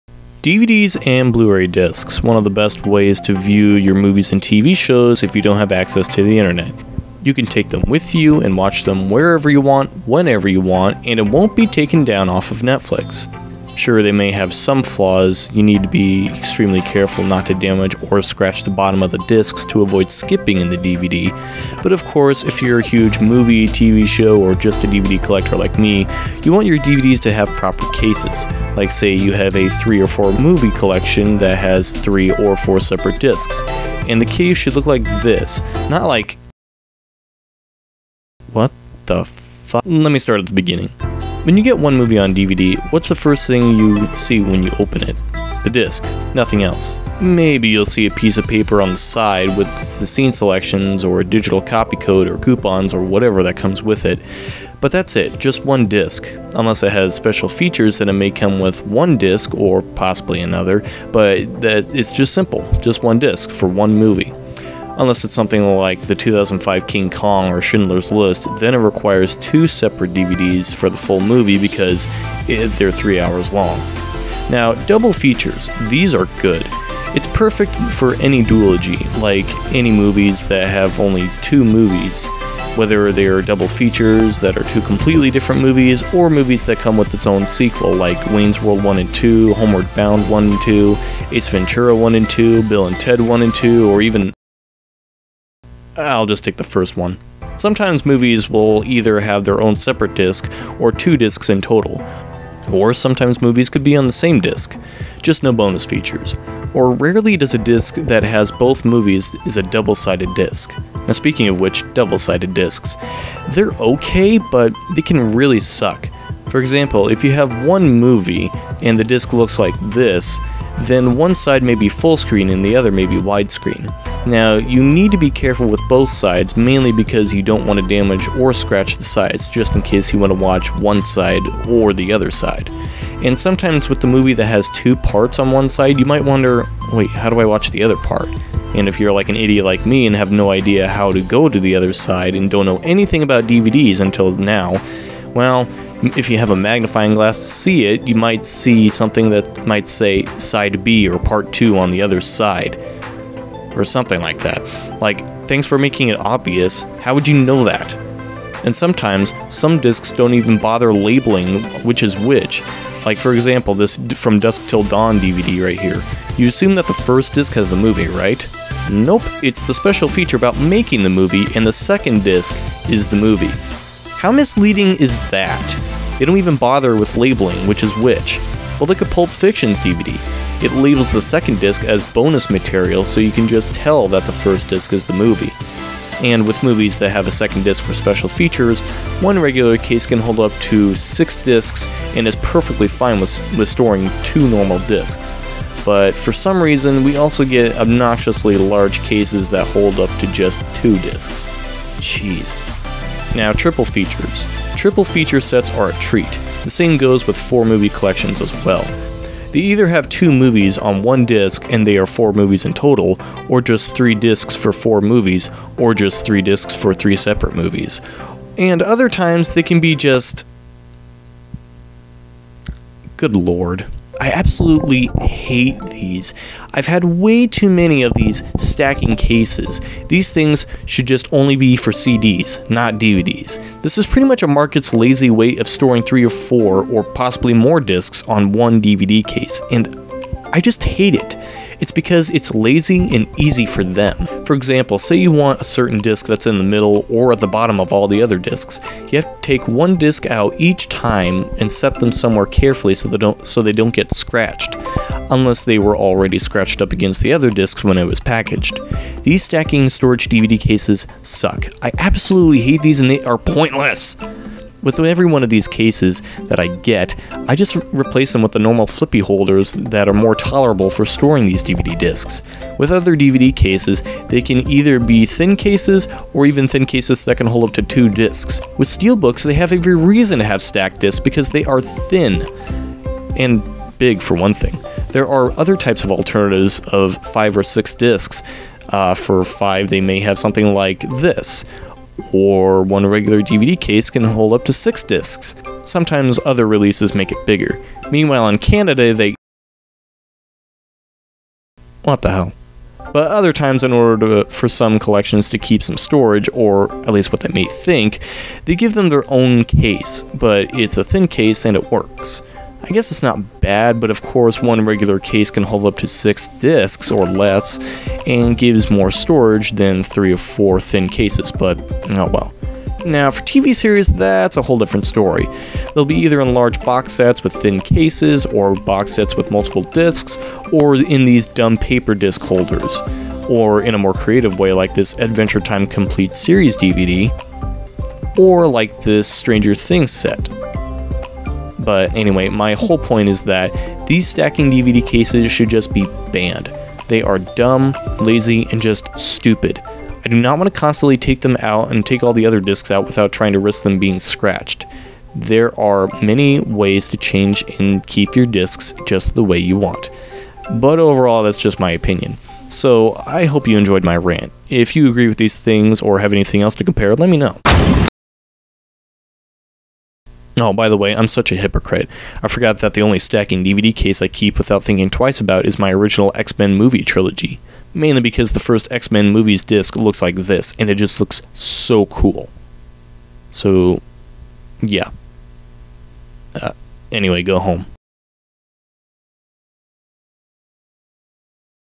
These DVD Cases NEED to be BANNED!!
This is one of my biggest complaints of these "Stackable DVD Cases". They are worthless.